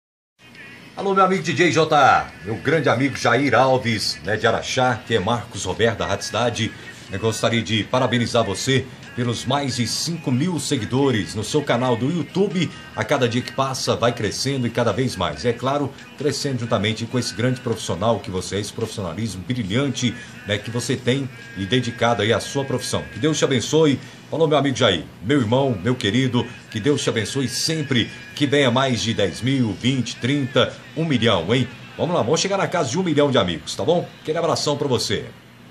5.000 inscritos (Meu irmão Marcos Roberto)
Marcos Roberto locutor experiente com mais de 30 anos de profissão. Trabalha na Rádio Cidade Fm Araxá. Obrigado meu irmão pelo carinho, 90% da minha Carreira eu dedico a você, pois vc sempre acreditou no meu trabalho e nunca deixou de me ajudar. Que Deus lhe abençoe sempre e cuide sempre de você!!!